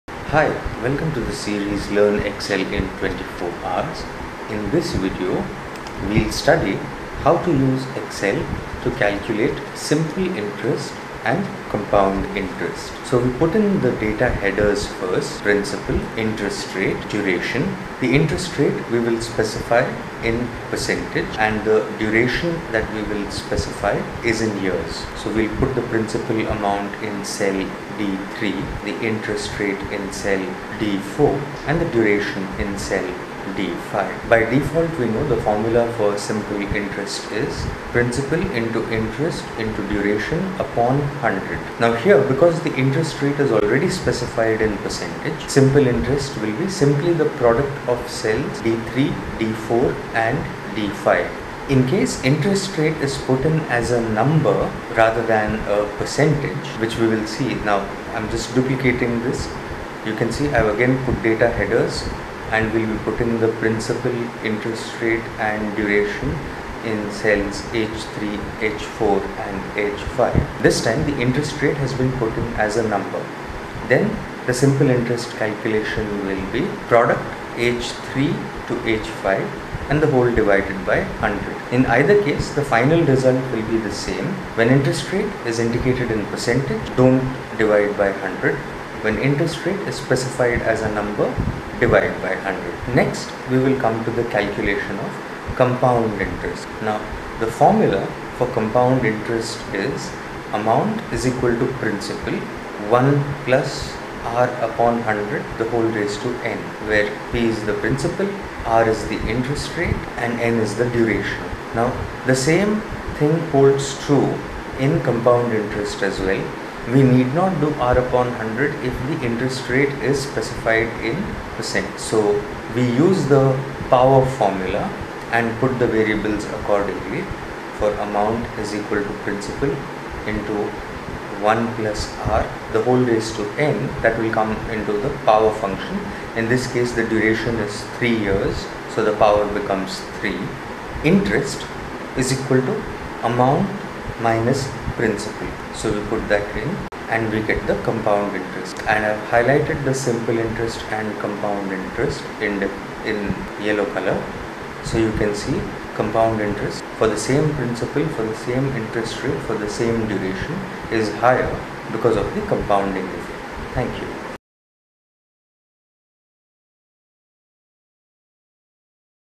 Excel Tutorial For Beginners - How To Calculate Simple & Compound Interest | MS Tools Tutorials
An easy way to calculate compound interest in Excel is to create a macro function. First start the Visual Basic Editor, which is located in the developer tab. Click the Insert menu, and click on Module. Then type "Function Compound_Interest(P As Double, i As Double, n As Double) As Double" in the first line.